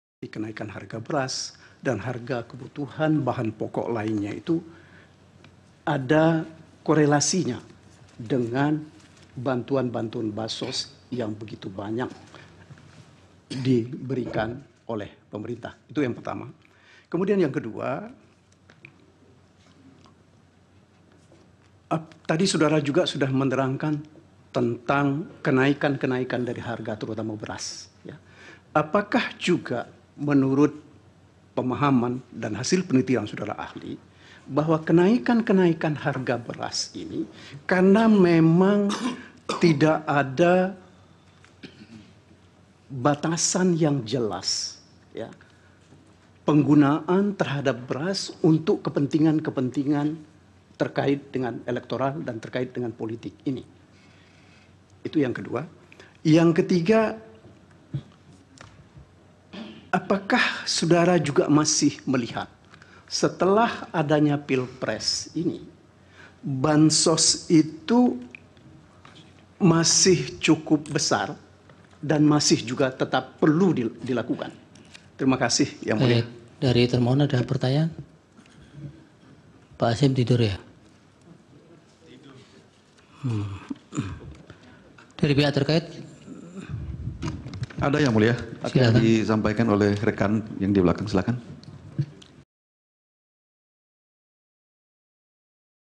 TIDUR SAAT SIDANG SENGKETA PILPRES, KETUA KPU DITEGUR KETUA MK
Ketua Mahkamah Konstitusi (MK), Suhartoyo, menegur Ketua KPU, Hasyim Asyari lantaran terlihat tertidur saat sidang lanjutan Perselisihan Hasil Pemilihan Umum (PHPU) Pilpres 2024.